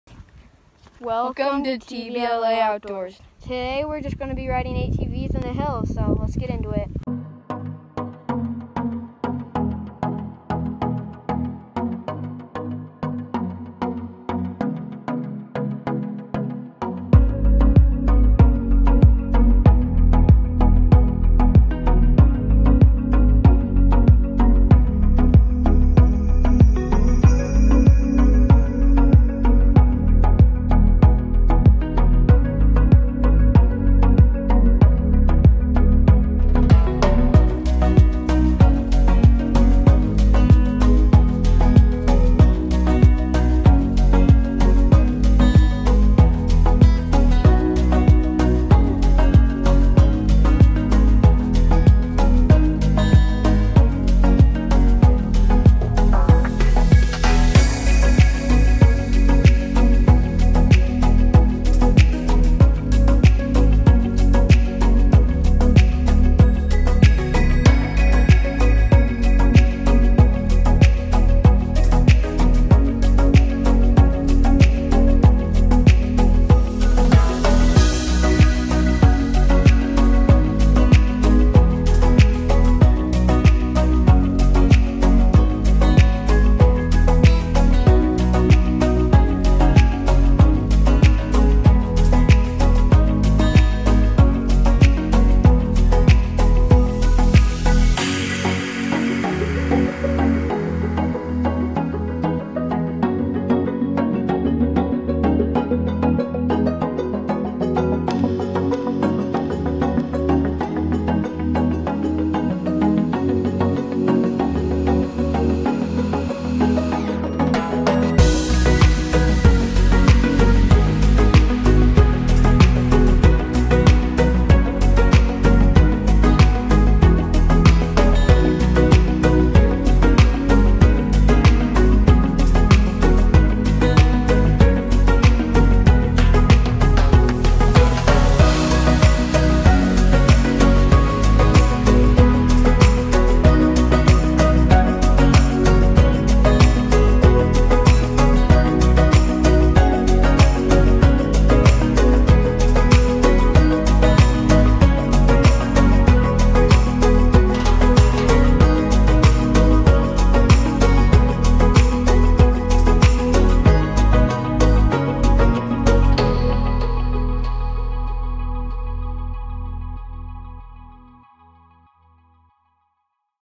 Riding atvs in the hills
In this video it’s just Layne and Brynlon riding the four wheeler in the hills.